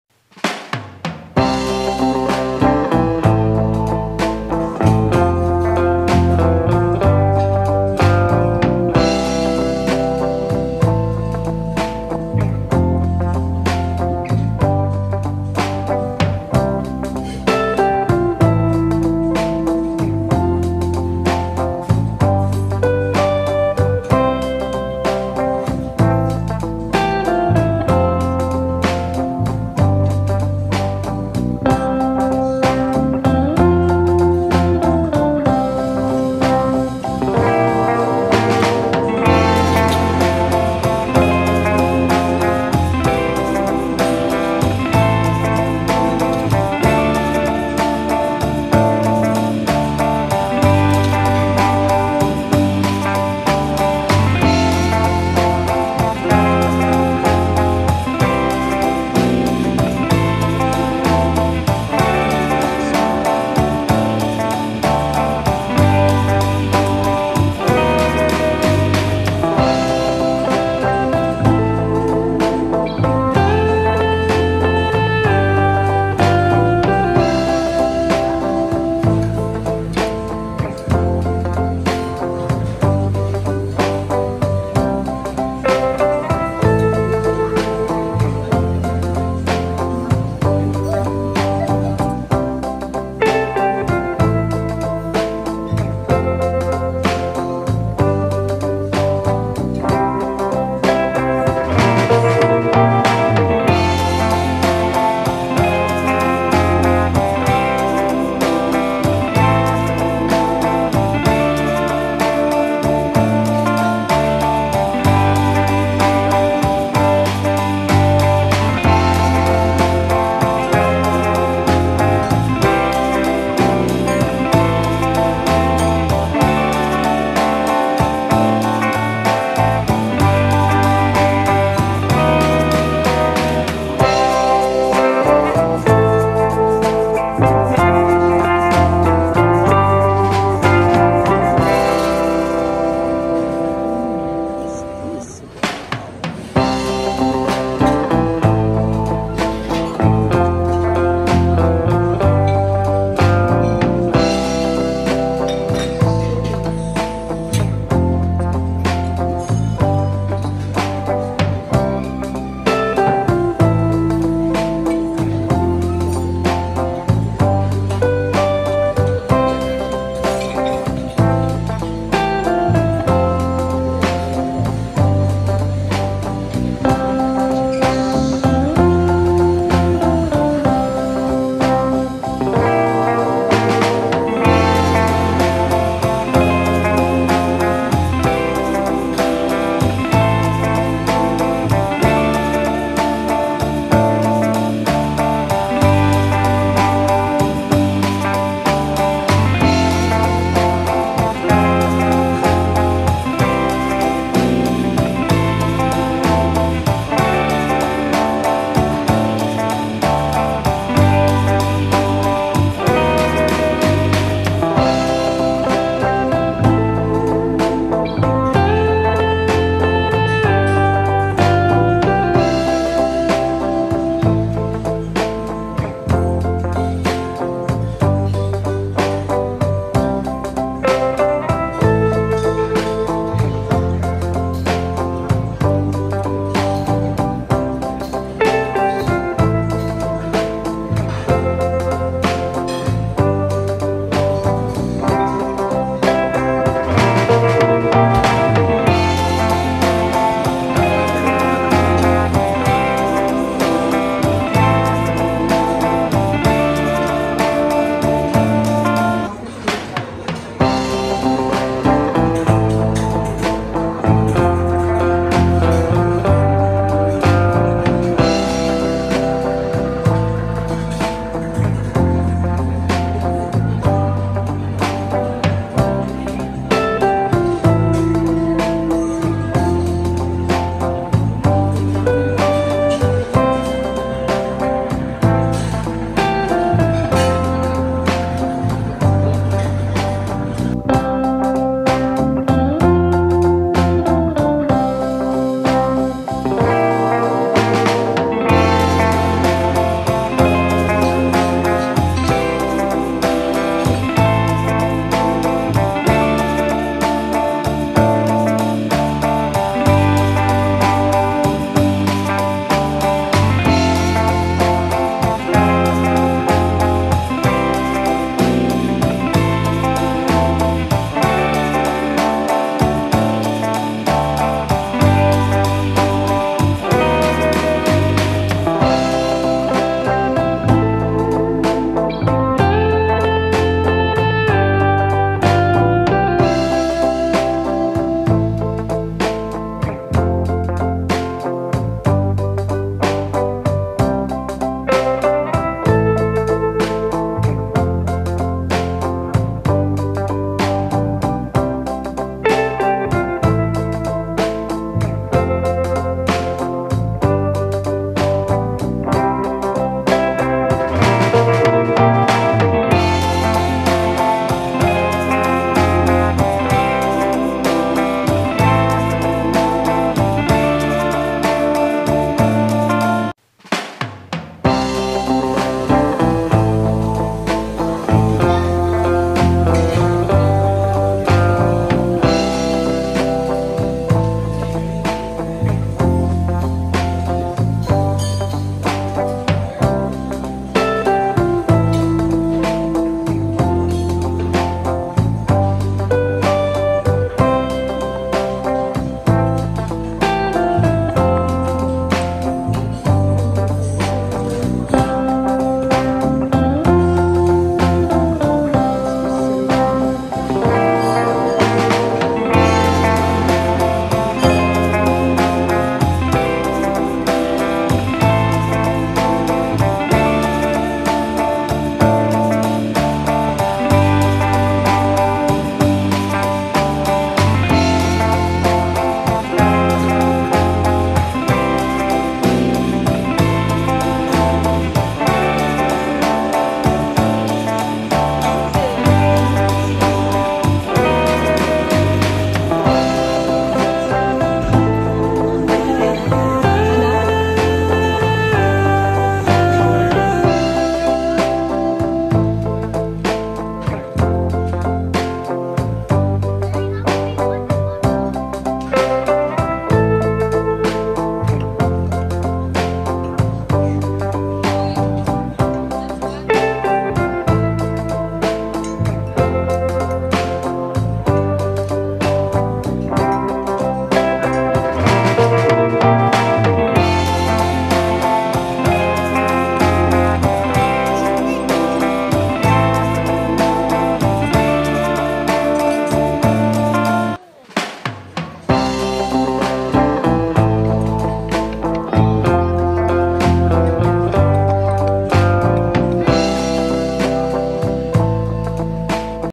BUFFET IN DUBAI🇦🇪@JW MARRIOT MARQUIS HOTEL#FAMILYDINNER
Will recomend JW MARRIOT MARQUIS HOTEL in dubai,guys the food so delicious specially the grilled station like steak, fish, chicken and a lot of veggies. They served international food!